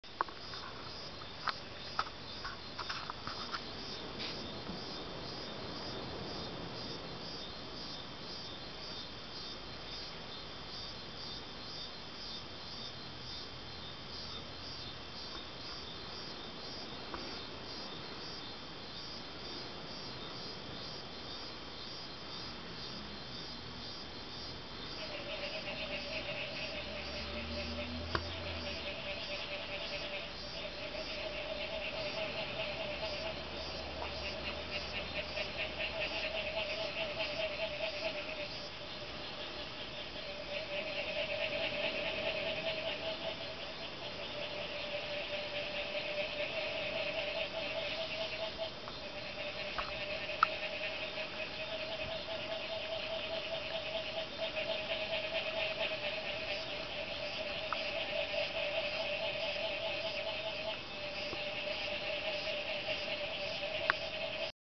Firefly's & Frogs view in full screen
28 firefly's and frogs on/in a black screen in Ocotal Bay, Guanacaste, Costa Rica.